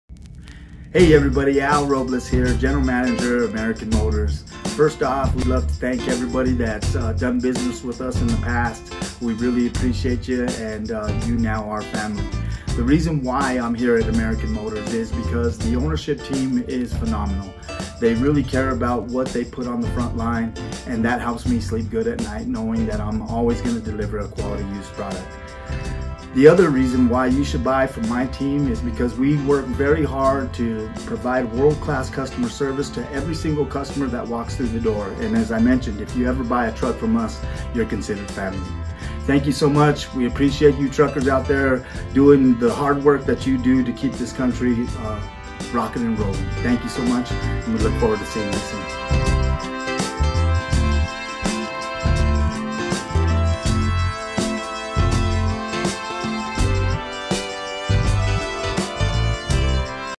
Meet The Team: Al Robles
Meet Al Robles everyone! Al is the General Manager of American Motors, Commerce City.